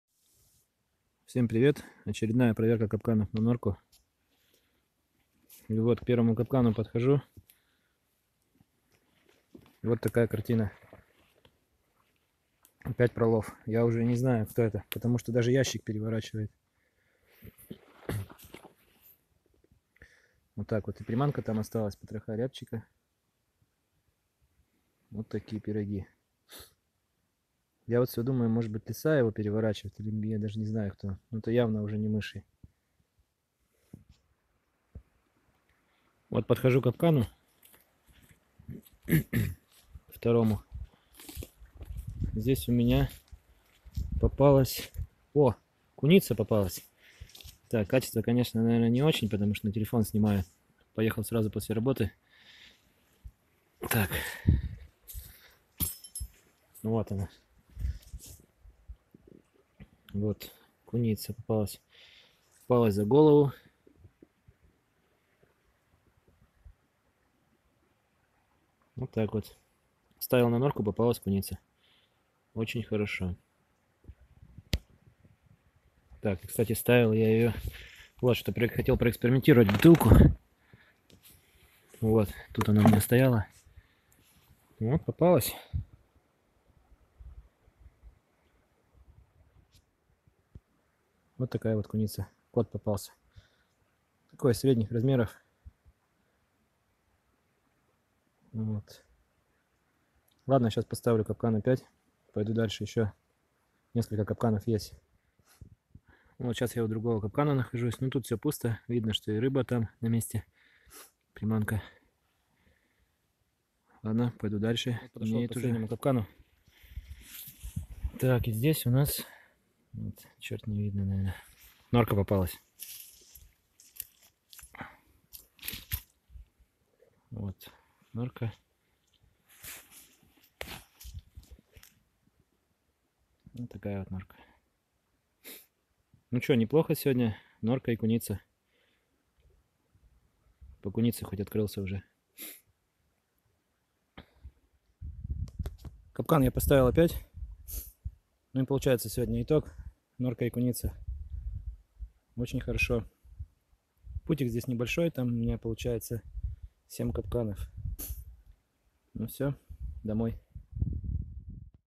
ОХОТА НА КУНИЦУ КАПКАНАМИ / КУНИЦА И НОРКА
Очередная проверка капканов на норку.Удача,попались куница и норка.